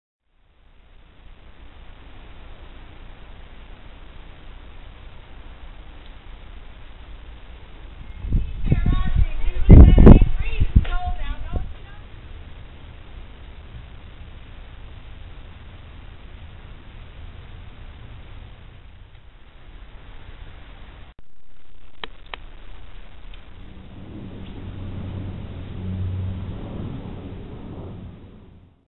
Henry Cowell Redwood Grove
Creatures great and small found in the Redwood Loop. Banana slug, squirrel, chickadee, people.